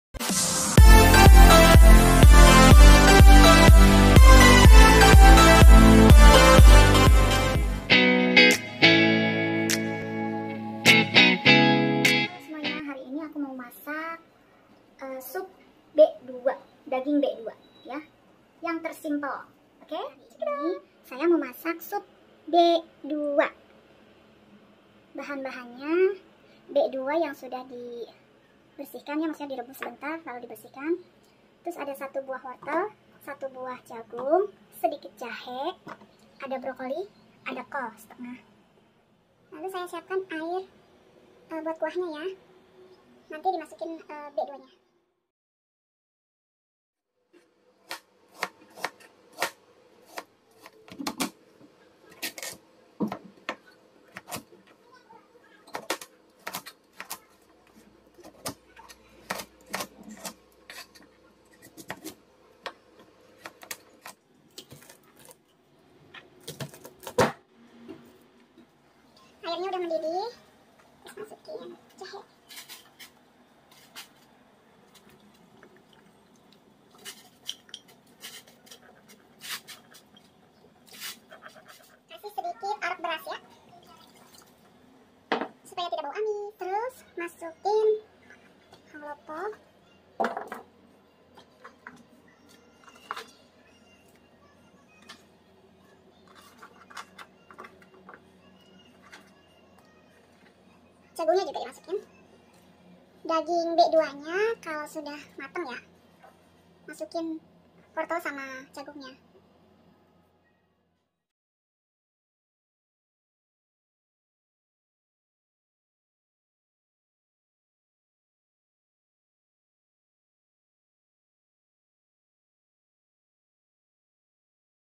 SUP DAGING B4BI 豬肉湯❗MASAKAN TAIWAN BUAT PEMULA || Praktis
Masak sup daging babi buat pemula yang sangat praktis mudah